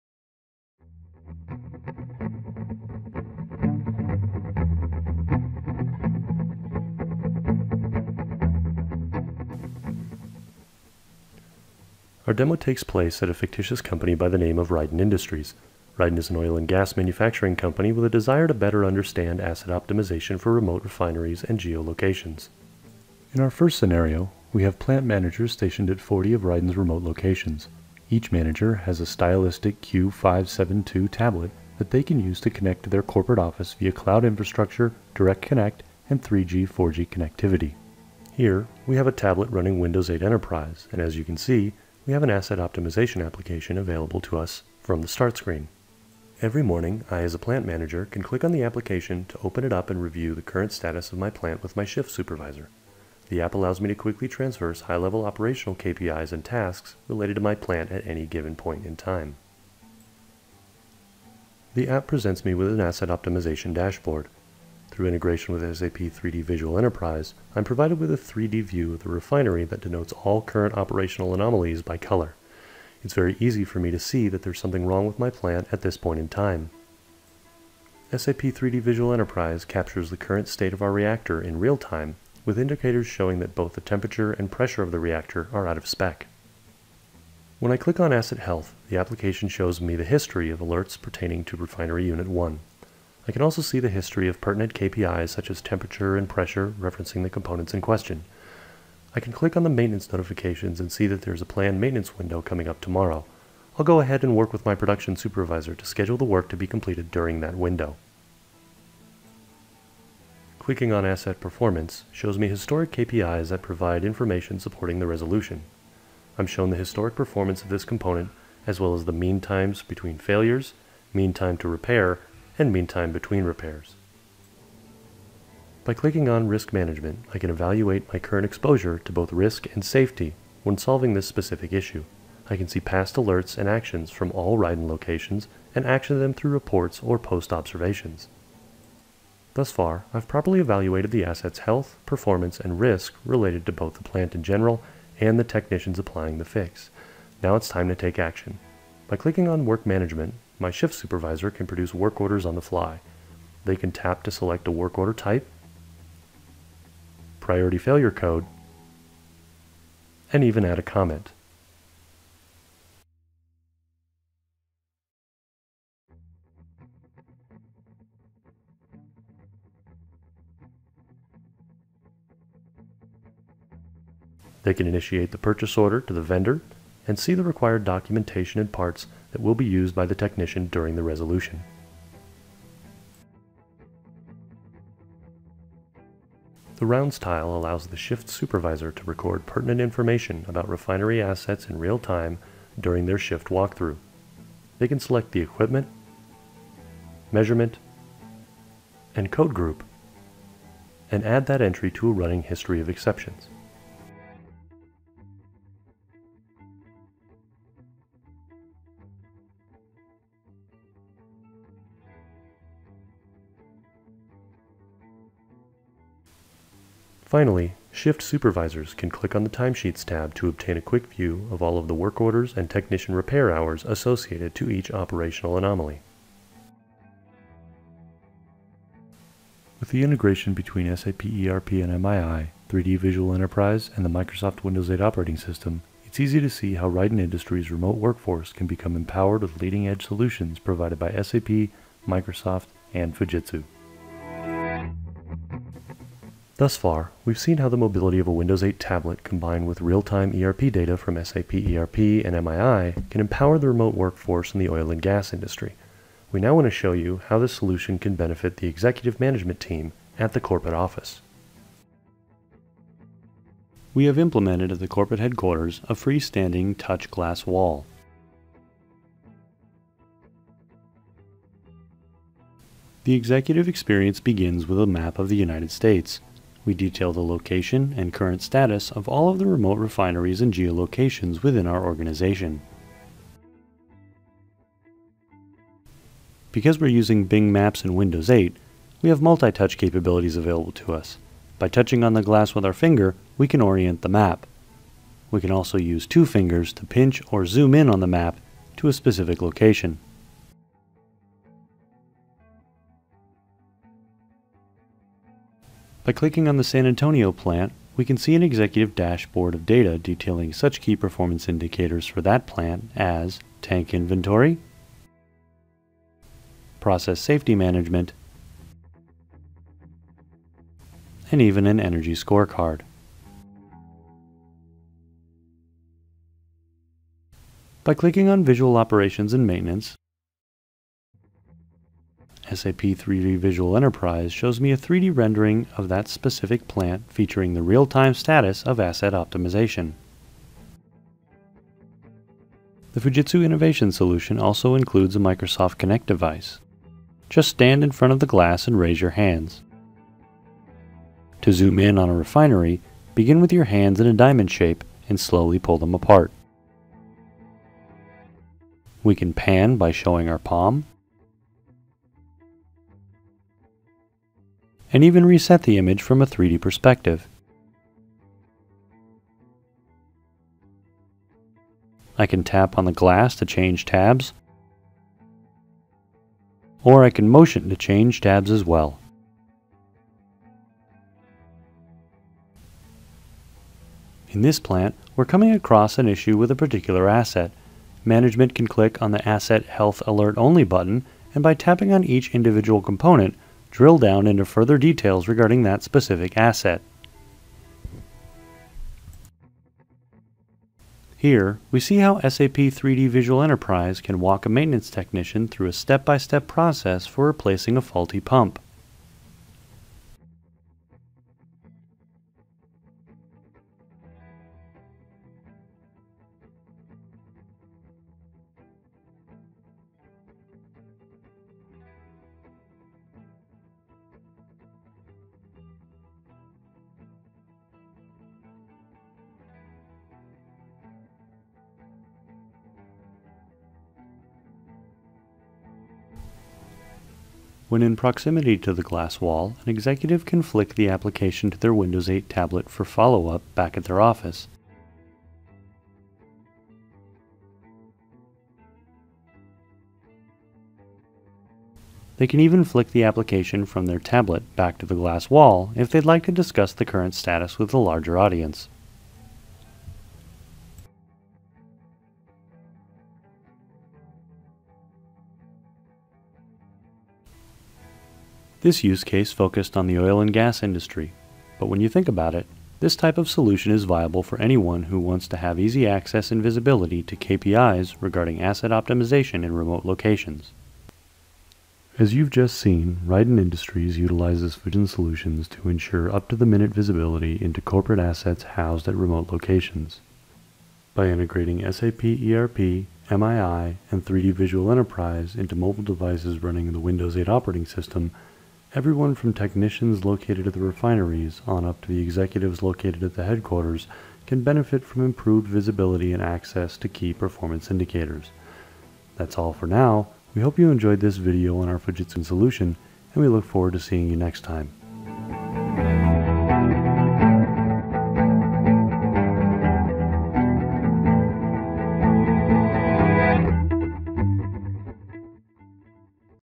Fujitsu Demo: SAP Operational Excellence at the Enterprise Level
Description: See an oil/gas manufacturing company demonstration providing real-time operation information on mobile and gesture controlled devices, SAP Rapid Deployment Solution of condition-based maintenance, asset analytics and mobile work manager.